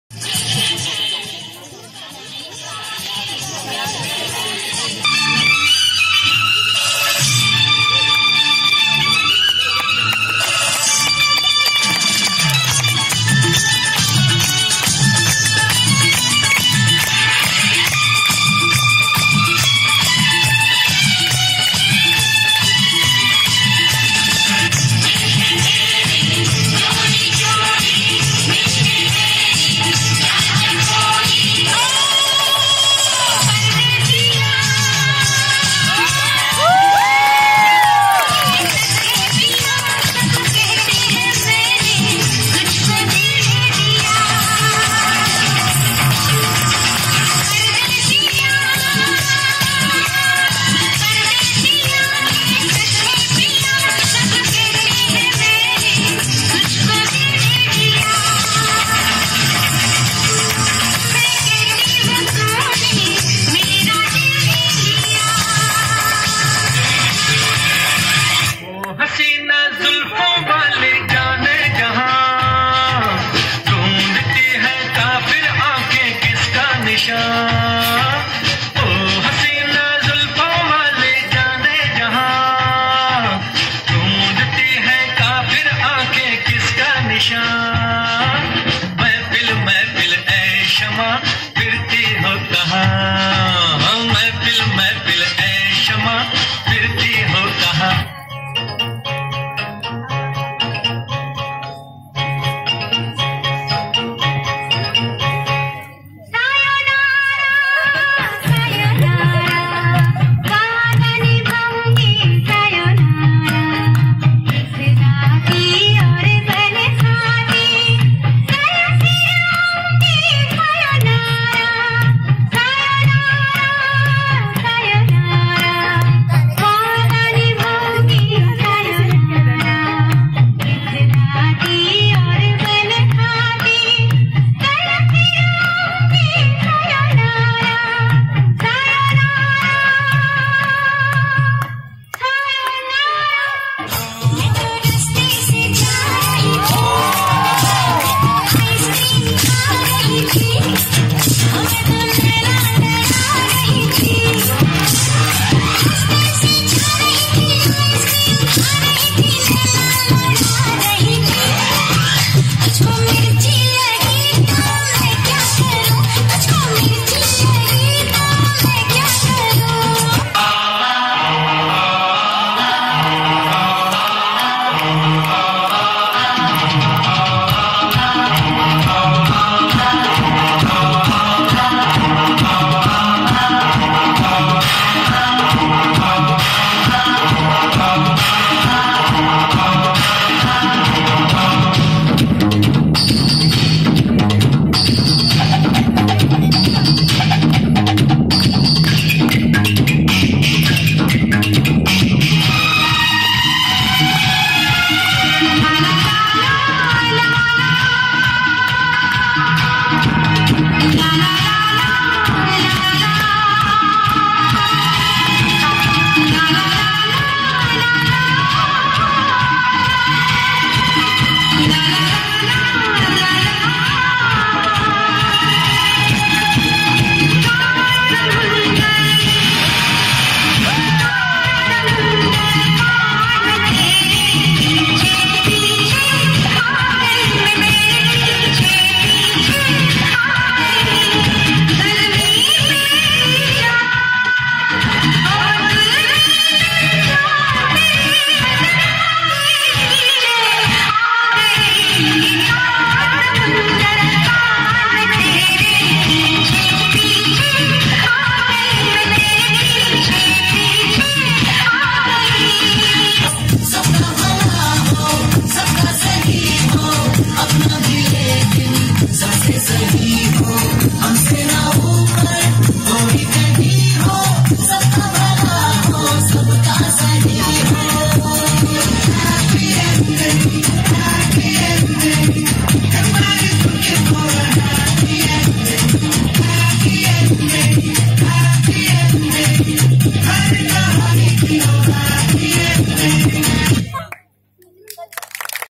Old song Dance || Anurag College Pharmacy,Warthi,Bhandara || In program 2018
Old song Dance || Anurag College Pharmacy,Warthi,Bhandara ||in annul program 2017-18 at Ramtake .
Perform by savi, prachiand their group.